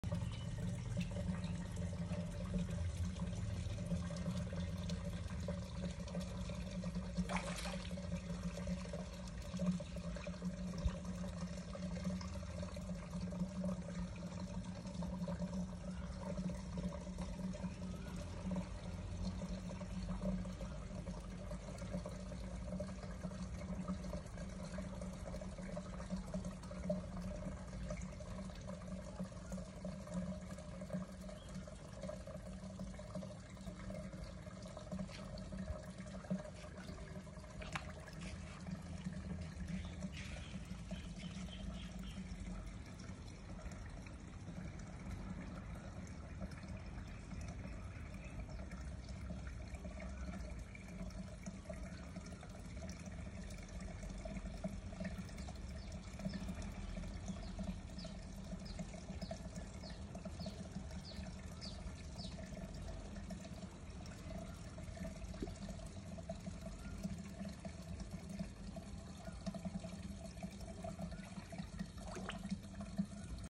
อัตเดต บ่อปลาแม่น้ำ วันที่ 31/03/2021 (ปลาช่อนอเมซอน เรดเทล อริเกเตอร์ ไทเกอร์เรดเทล กระเบน ชะโด )
31/03/2021 หลังจากเพิ่มหลอดuv เป็น 136 วัต และเพิ่มปลาซักเกอร์อีก 6ตัว ทำให้บ่อปลาตะไคร่หายไปอย่างเห็นได้ชัดครับ
ปลาในบ่อ
- ช่อนอเมซอน
- อริเกเตอร์
- เรดเทล
- ไทเกอร์เรดเทล
- ไทเกอร์โชวโนส
- ชะโด
- เทพา
- ช่อนข้าหลวง
- กระเบน
- ดุกหนามไนเจอร์
- ยีราฟแคชฟิช
- บีเซีย